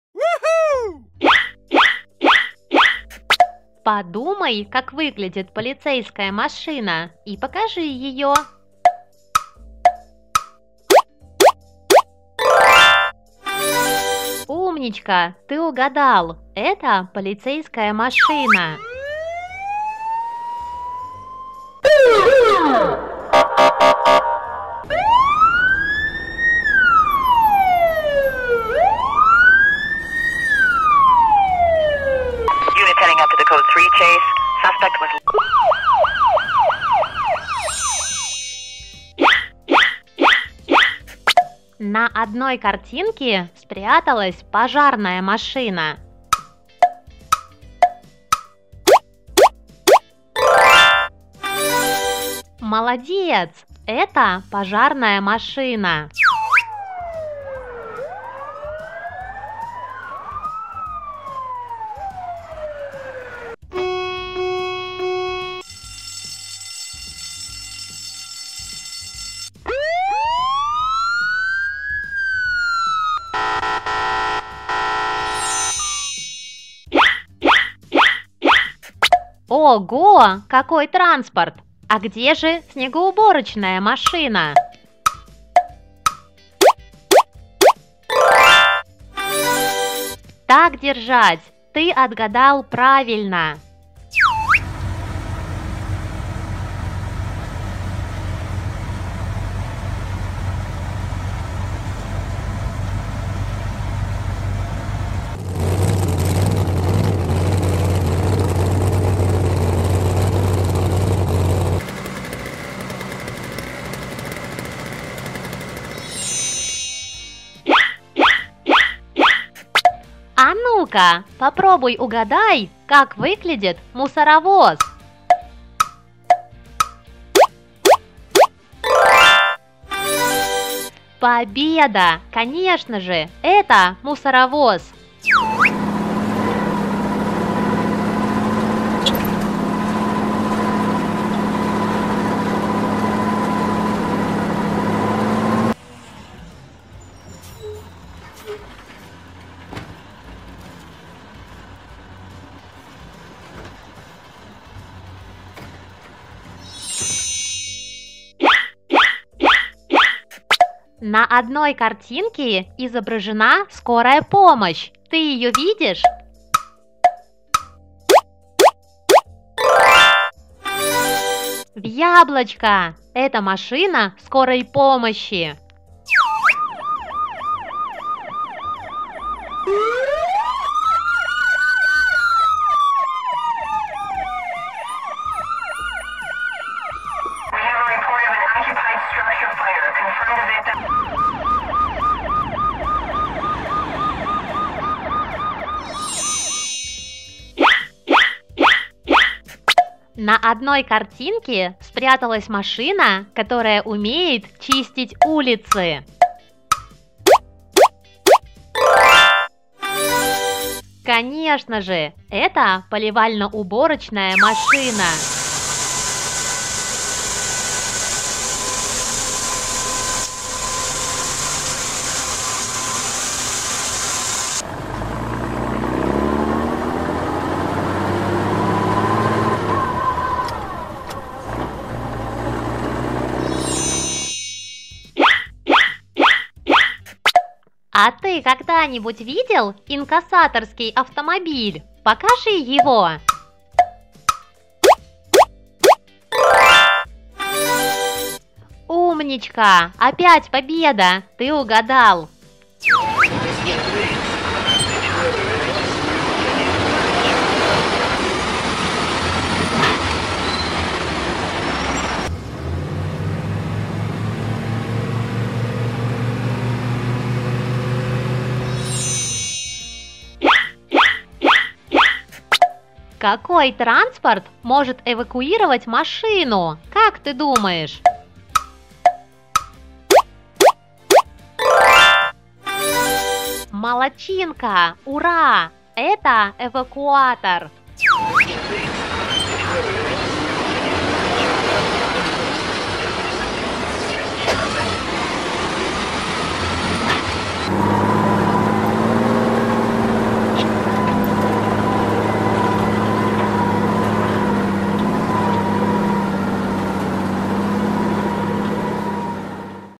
Машины с мигалками для детей. Служебный транспорт для малышей. Мультик про машинки и спец технику.
Мультик про  машины  с мигалками для детей. Развивающее видео для малый о служебном транспорте и машинках специального назначения .